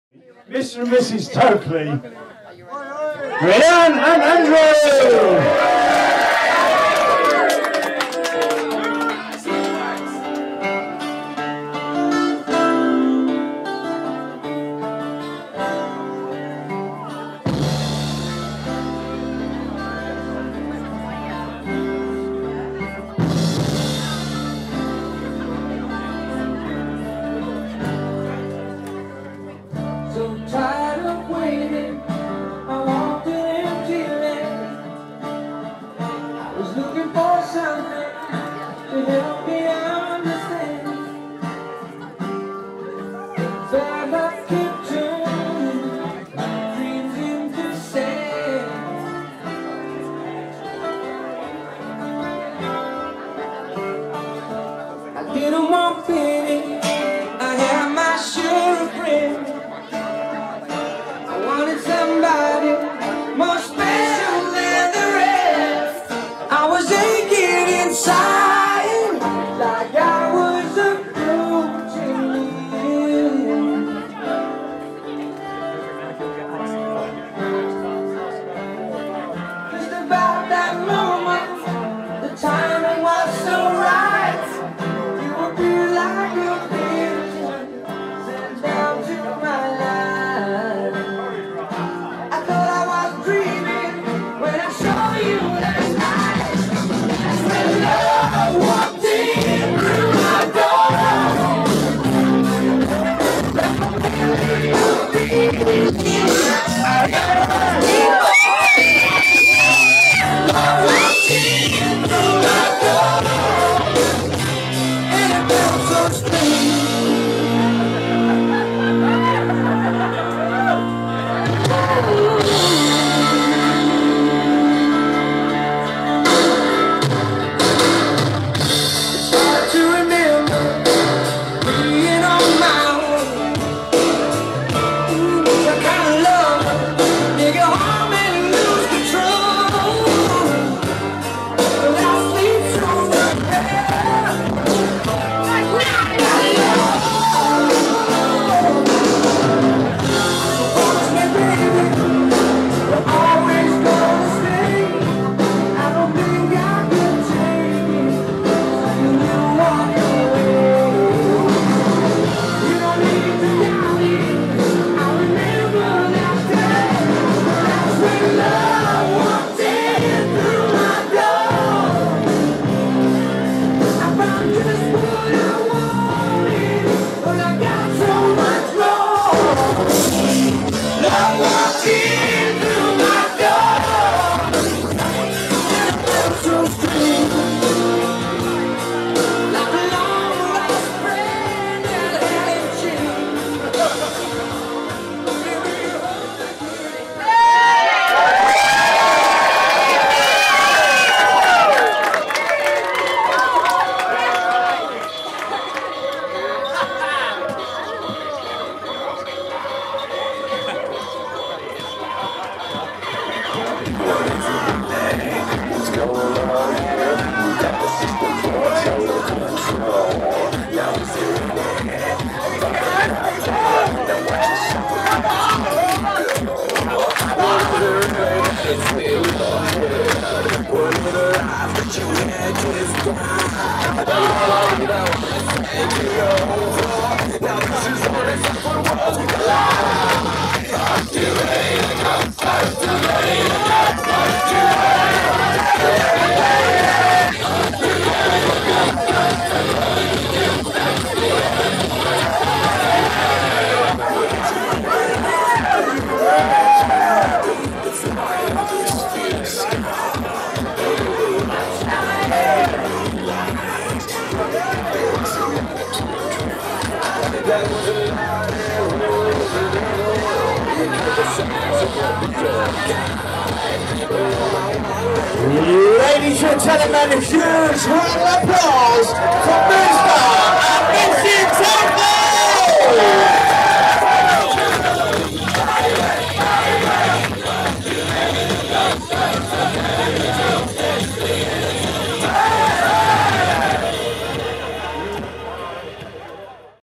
Rhian & Andrew rocking first wedding dance & mosh
Very Rock & Roll and our guets join in for a mosh pit!